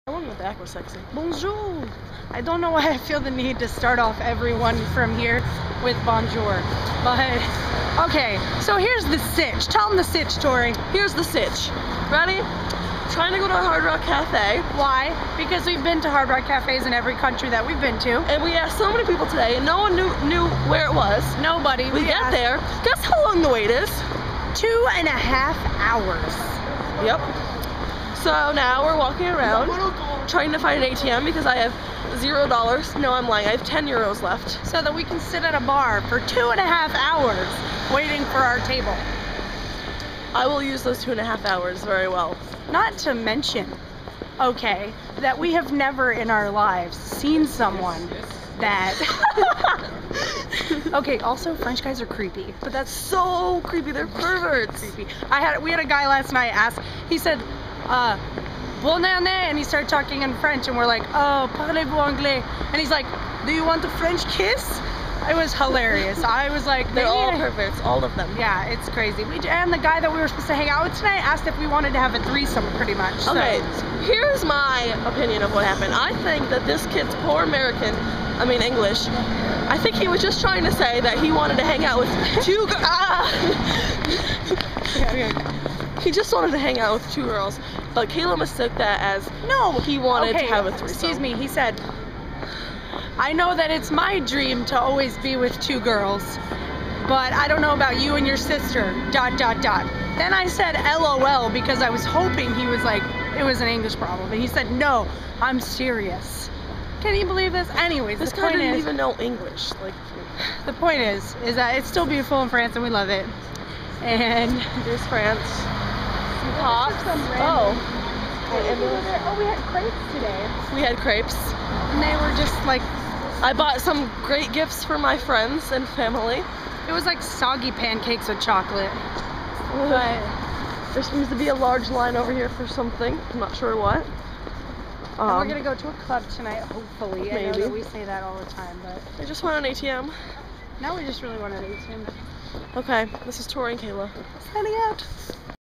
Video 7 - 2.5 hour wait for the Hard Rock Cafe - Paris
2.5 hour wait for the Hard Rock, trying to find an ATM and, oh yeah, French guys are apparently creepy.